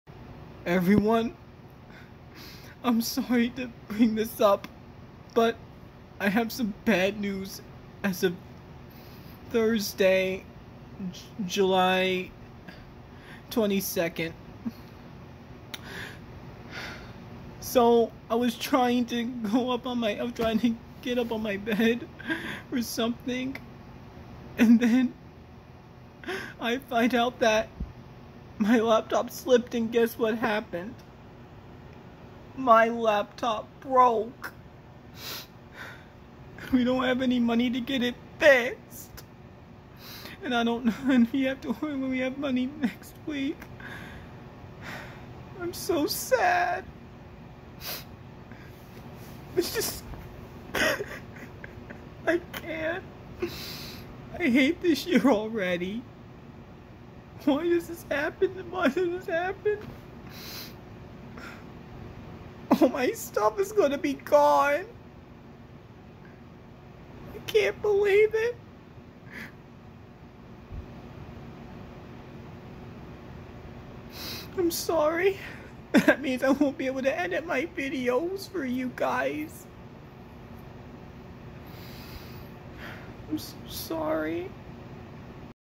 bad news you all won't believe what just happened
my screen cracked on my laptop